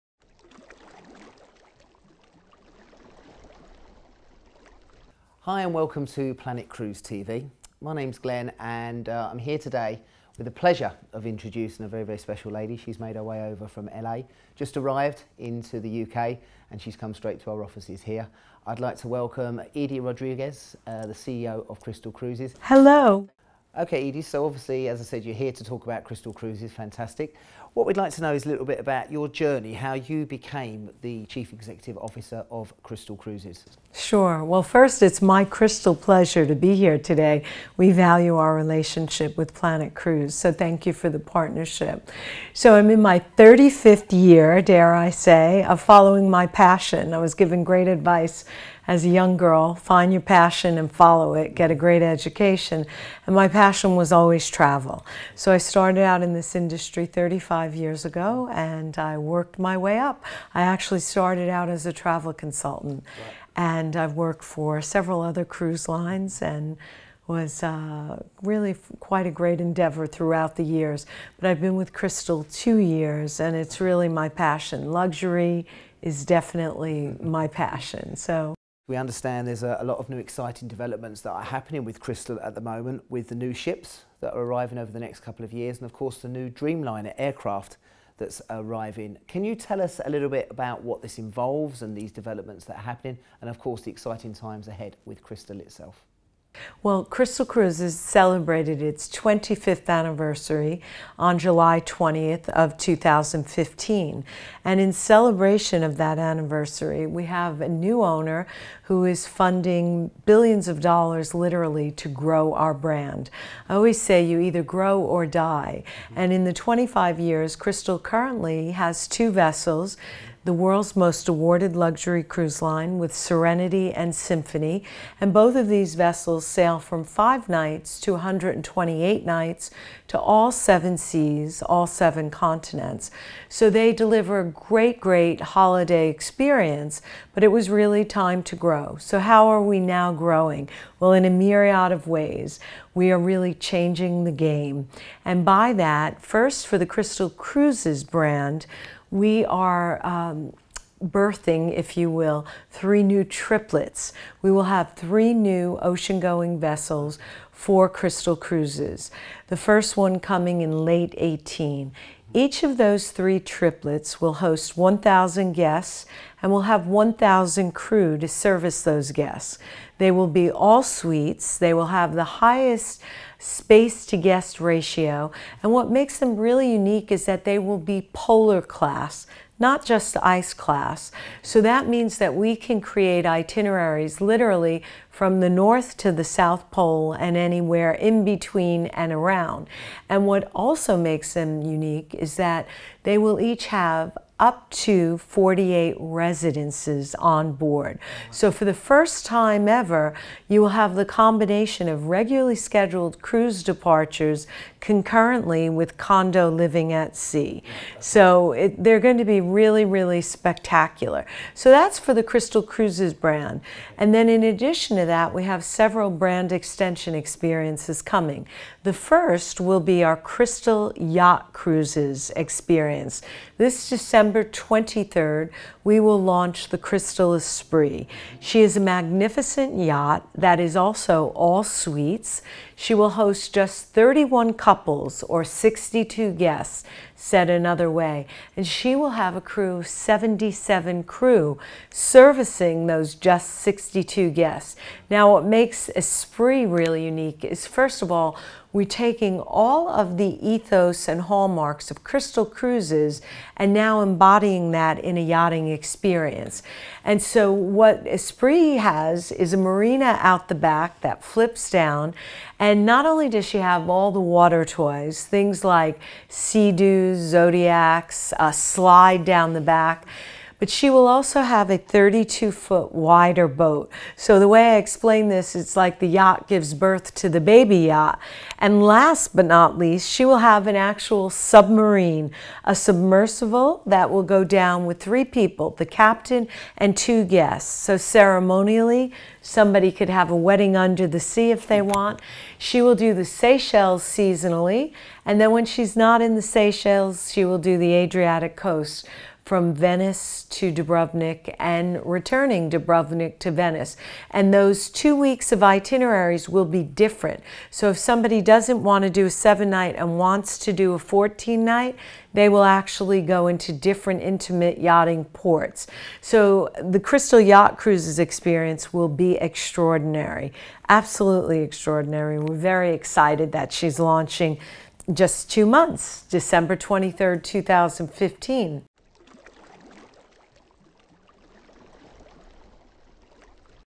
Edie Rodriguez Interview, CEO Crystal Cruises | Planet Cruise
We got the chance to catch up with, CEO of Crystal Cruises, Edie Rodriguez about her amazing career and the new products coming  in the next few years from Crystal Cruises.